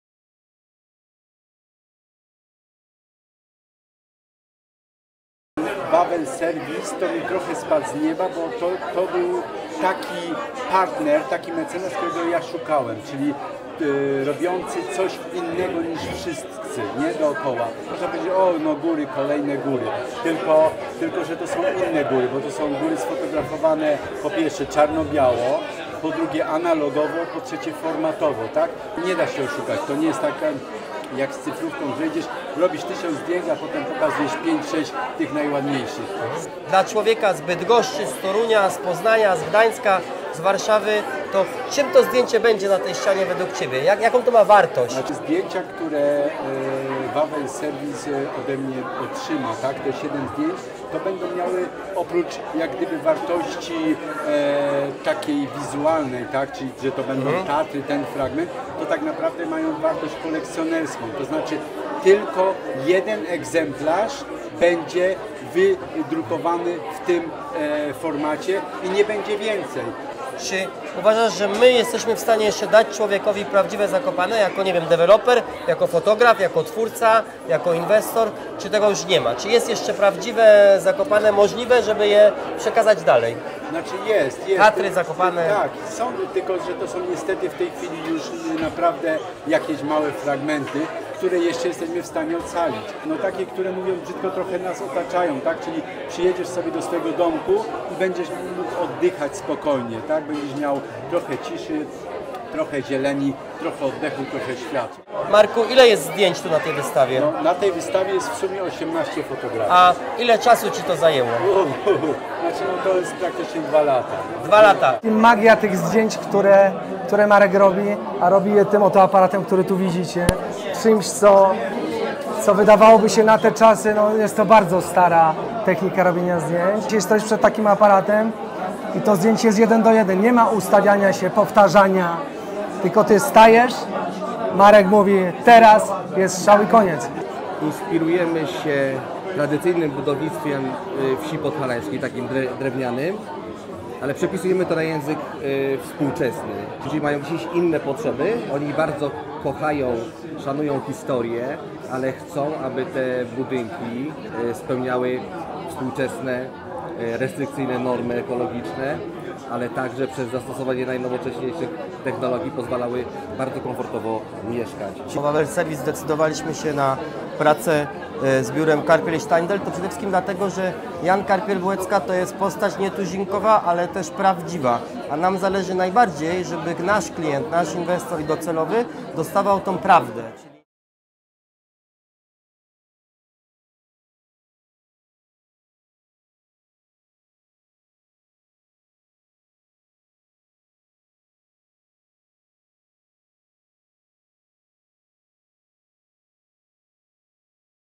7 unikalnych zdjęć trafi do nabywców domów inwestycji Czajki Prestige w Kościelisku
Wawel Service po raz kolejny postanowił wesprzeć artystyczną inicjatywę i objąć mecenatem wystawę "Tatry Wielkoformatowo" autorstwa Marka Kowalskiego.
7 unikalnych i niepowtarzalnych zdjęć z tej wystawy trafi do nabywców wyjątkowych domów inwestycji Czajki Prestige.
To sztuka inspirowana górami, pokazuje idee, prostotę i ducha gór. Nas jako dewelopera, również góry inspirują, aby tworzyć rzeczy piękne, użyteczne i przekazać odbiorcy dokładnie to samo co Marek chciał przekazać poprzez swoje fotografie.
Staramy się aby wszystko co robimy miało wysublimowany smak regionu, dlatego też tak chętnie współpracujemy z twórcami i artystami bezpośrednio powiązanymi z miejscem, w którym budujemy.